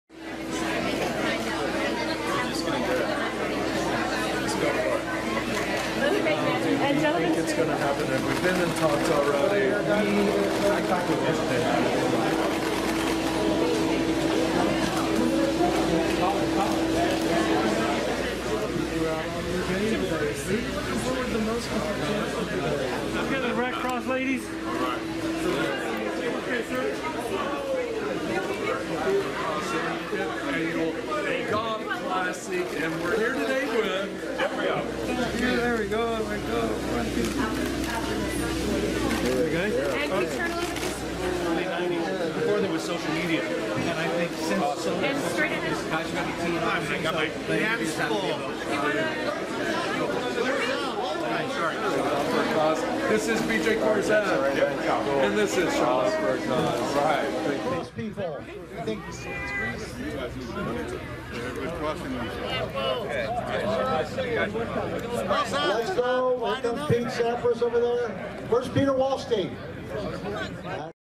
EVENT CAPSULE CLEAN - Red Cross' 5th Annual Celebrity Golf Tournament - Arrivals
EVENT CAPSULE CLEAN - Red Cross' 5th Annual Celebrity Golf Tournament - Arrivals on April 16, 2018 in Burbank, California.

Featuring:
    Breckin Meyer
    Mark-Paul Gosselaar
    James Remar
    Jeffrey Osborne
    Kurt Fuller
    Michael Chiklis
    Nick Swisher
    Richard Karn
    Robert Herjavec